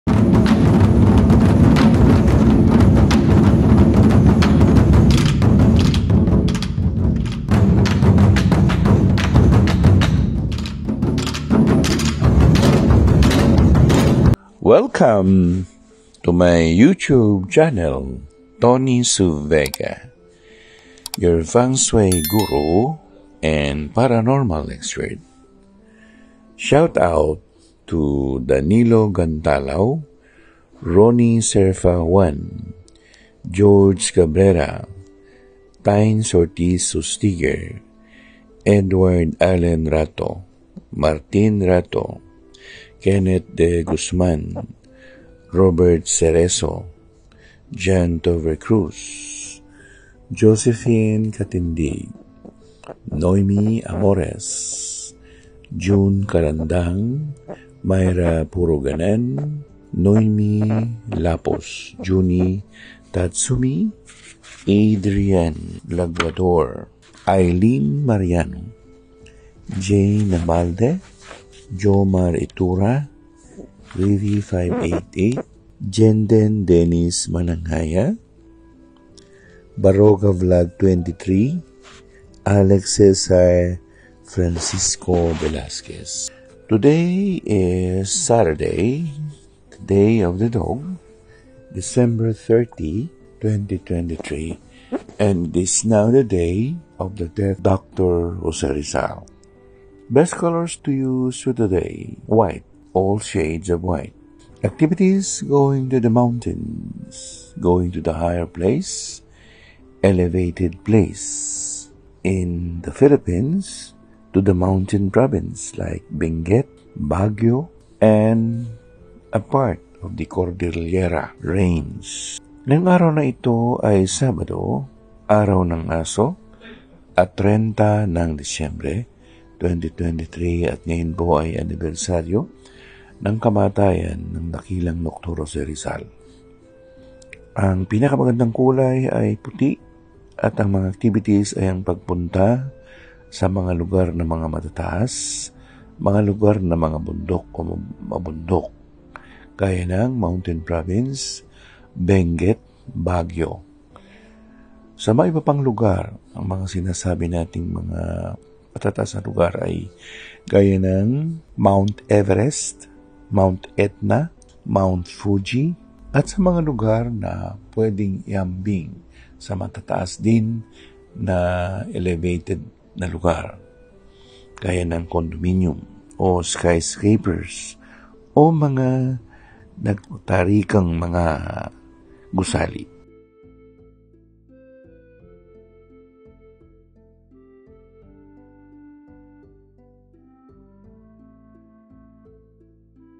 30 December 2023; Saturday, Day of the Dog
0:00 Intro
0:26 Shoutout Portion
1:26 Lucky things to do for today (English)
2:02 Mga gagawin para maging maswerte ngayong araw (Filipino)
3:02 Outro

I specializes in:
• Feng Shui
• Tarot Reading
• Paranormal
• Crystals and Gemstones
• Lucky charms
• Angelology
• Astrology